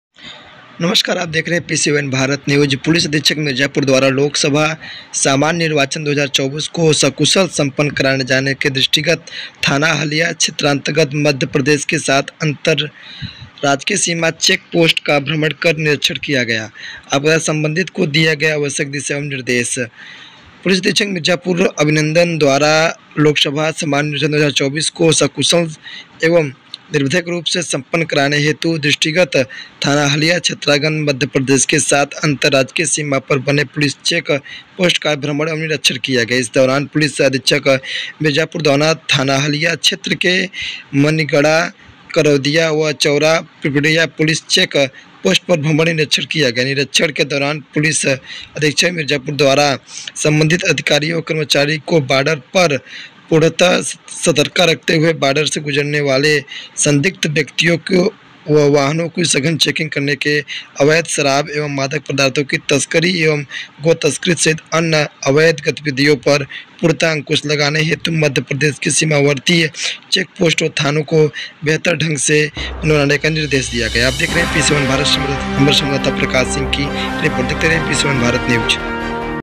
पुलिस अधीक्षक मीरजापुर "अभिनन्दन" द्वारा किया गया रूटमार्च
*मीरजापुर पुलिस*
*—प्रेस नोट—*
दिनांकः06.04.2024
*पुलिस अधीक्षक मीरजापुर द्वारा लोकसभा सामान्य निर्वाचन-2024 को सकुशल सम्पन्न कराये जाने के दृष्टिगत थाना हलिया क्षेत्रान्तर्गत मध्य प्रदेश के साथ अन्तर्राज्यीय सीमा चेक पोस्ट का भ्रमण/निरीक्षण कर सम्बन्धित को दिया गया आवश्यक दिशा-निर्देश—*
                      आज दिनांकः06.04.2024 को पुलिस अधीक्षक मीरजापुर "अभिनन्दन" द्वारा लोकसभा सामान्य निर्वाचन-2024 को सकुशल एवं निर्बाध रुप से सम्पन्न करायें जाने के दृष्टिगत थाना हलिया क्षेत्रान्तर्गत मध्य प्रदेश के साथ अन्तर्राज्यीय सीमा पर बने पुलिस चेक पोस्ट का भ्रमण/निरीक्षण किया गया । इस दौरान पुलिस अधीक्षक मीरजापुर द्वारा थाना हलिया क्षेत्र के मनिगढ़ा, करौंदिया व चौरा पीड़रिया पुलिस चेक पोस्ट का भ्रमण/निरीक्षण किया गया । निरीक्षण के दौरान पुलिस अधीक्षक मीरजापुर द्वारा सम्बन्धित अधिकारी/कर्मचारीगण को बॉर्डर पर पूर्णतया सतर्कता रखते हुए बॉर्डर से गुजरने वाले संदिग्ध व्यक्तियों व वाहनों की सघन चेकिंग करने तथा अवैध शराब एवं मादक पदार्थों की तस्करी, गो-तस्करी सहित अन्य अवैध गतिविधियों पर पूर्णतः अंकुश लगाने हेतु मध्य प्रदेश के सीमावर्ती चेक पोस्ट व थानों से बेहतर ढंग से समन्वय बनाकर कार्यवाही करने के निर्देश दिए गये । चेक पोस्ट पर ड्यूटी के दौरान सम्बन्धित को नियमित चेकिंग सम्बन्धित विभिन्न प्रकार के रजिस्टर रखने तथा चेकिंग सम्बन्धित प्रविष्टियों को अद्यावधिक रखने के निर्देश दिए गये ।
                      उक्त अन्तर्राज्यीय बॉर्डर पर बने चेक पोस्ट के भ्रमण/निरीक्षण के दौरान अपर पुलिस अधीक्षक ऑपरेशन ओम प्रकाश सिंह, क्षेत्राधिकारी लालगंज शैलेन्द्र प्रकाश त्रिपाठी, थानाध्यक्ष हलिया विष्णु प्रभा सिंह सहित पुलिस के अन्य अधिकारी/कर्मचारीगण उपस्थित रहें ।